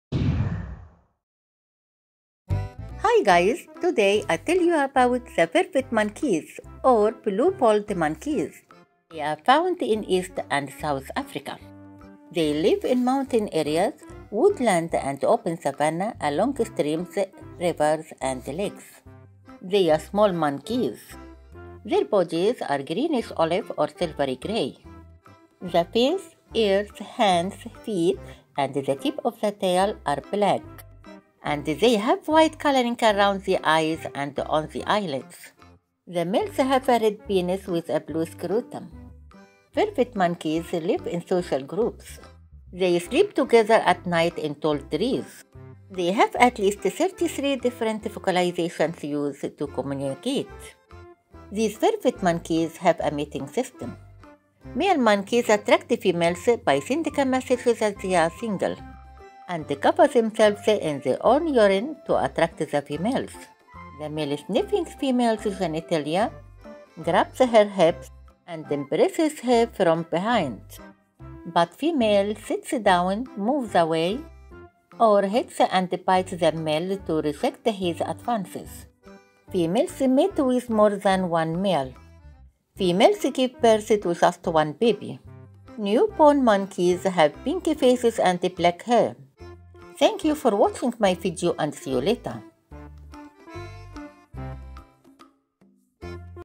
Vervet Monkey (Blue Balls) Mating Behavior
I'll tell you about (The vervet monkeys) or (blue balled monkeys). They live in
East and south Africa. They are small monkeys. They have a mating system.
They have at least 33 different vocalizations.